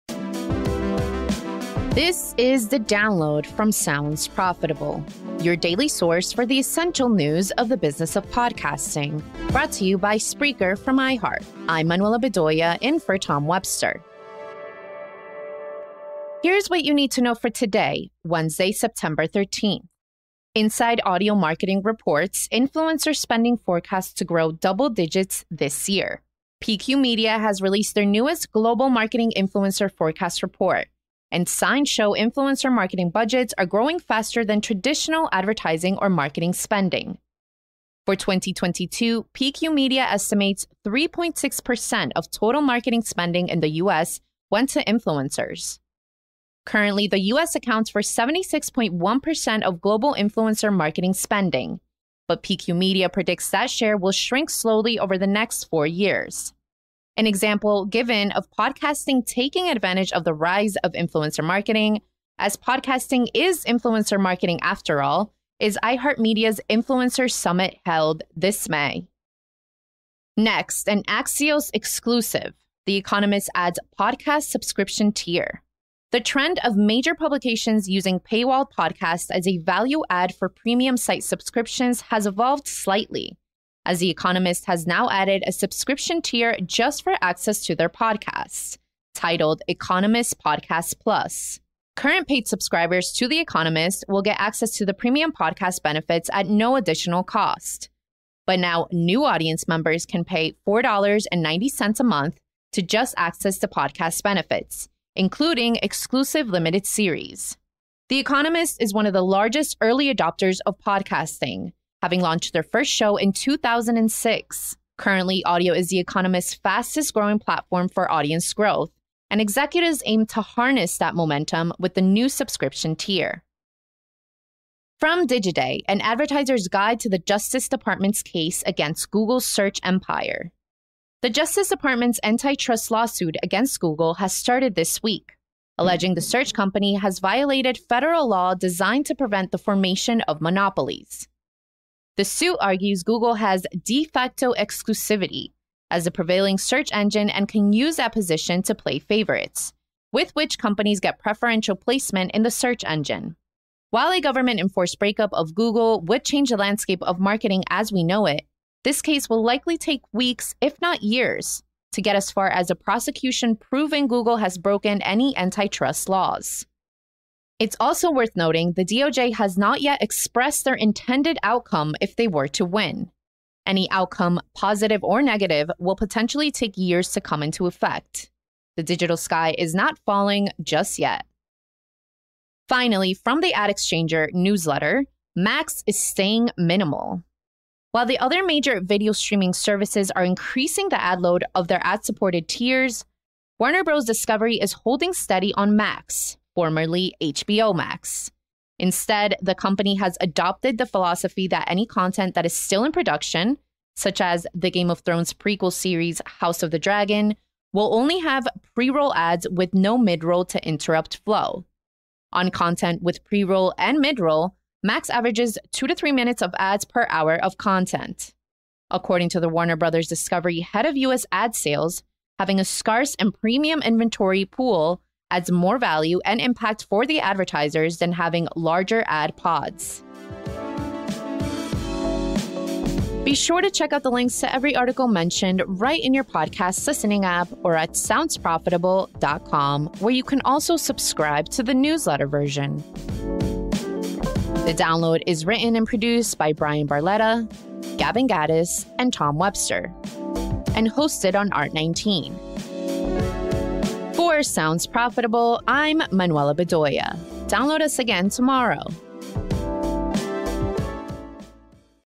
Podcasts Command Attention, Influencer Spend Increasing, the DoJ's Case Against Google & More.
Here's what you need to know for today in the business of podcasting:

[Influencer Spending Forecast To Grow Double-Digits This Year by Inside Audio
forecast-to-grow-double-digits-this-year)

tier)

[An advertiser’s guide to the Justice Department’s case against Google’s
guide-to-the-justice-departments-case-against-googles-search-empire/)